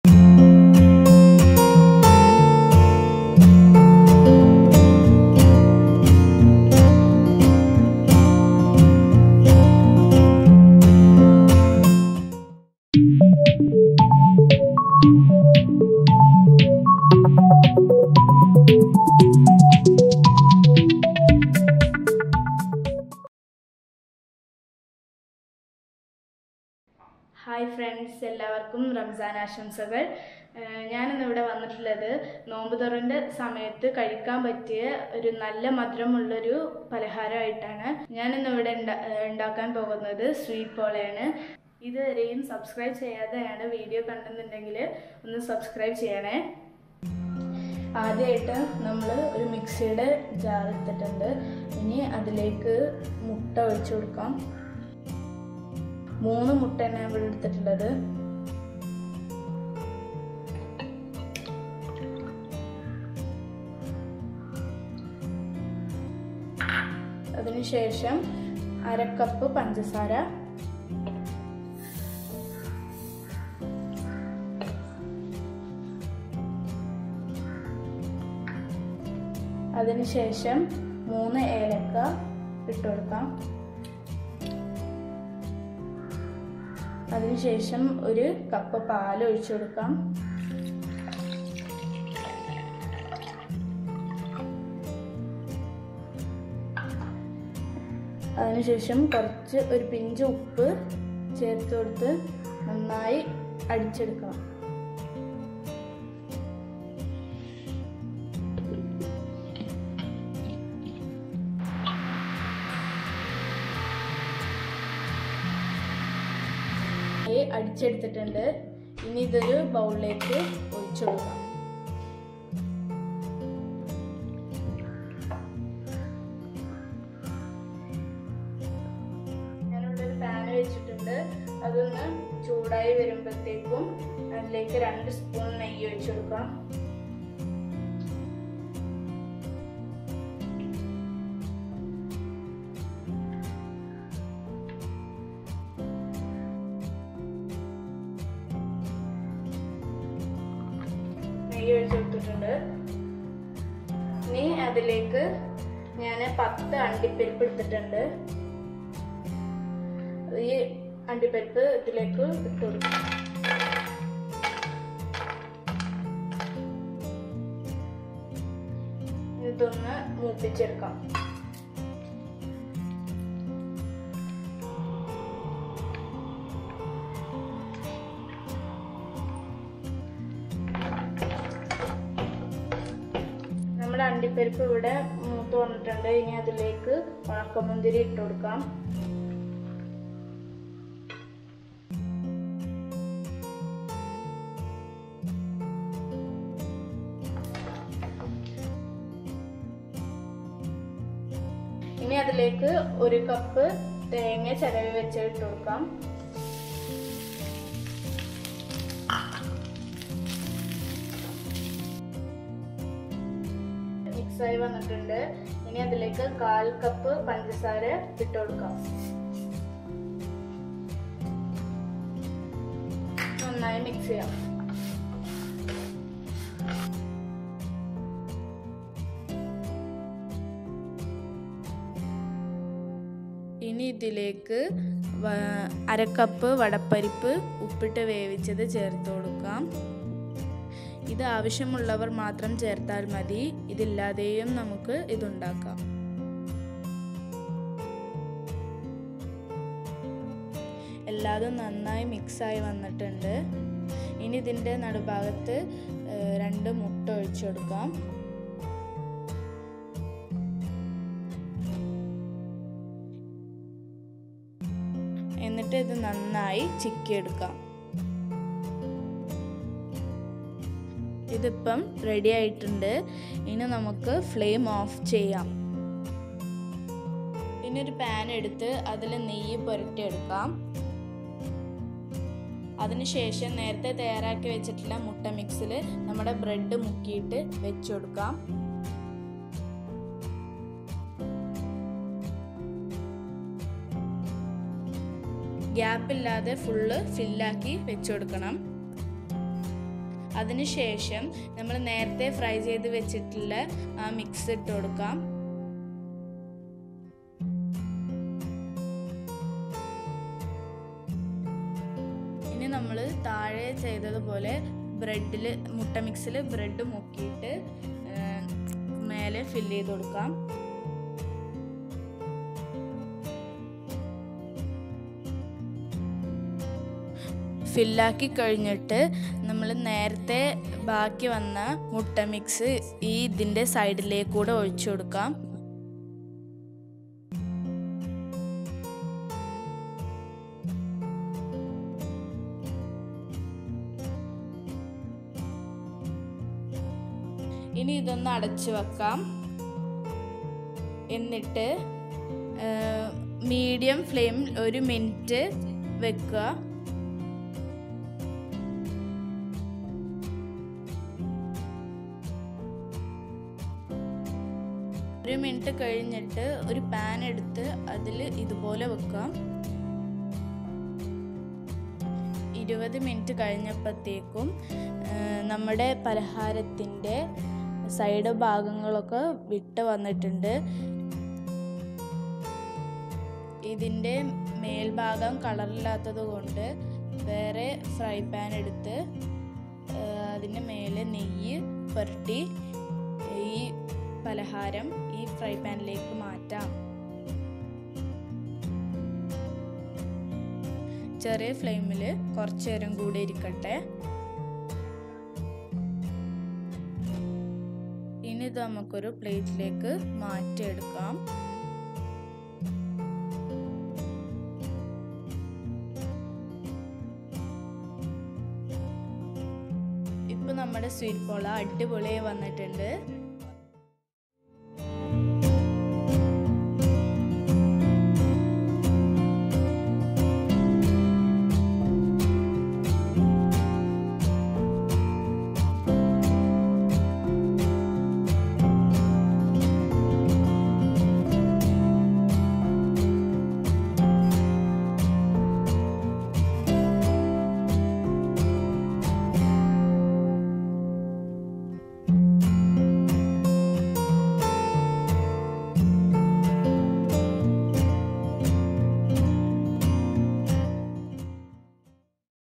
Sweet Pola | Ramzan Special | Leisures of Mehvish | Vlog 5
Ingredients:
Egg-5, Sugar Half Cup, Milk 1 Cup, Cardamom, Salt, Daal Half Cup, Coconut 1 Cup, Cashew, Raisins, Bread , Ghee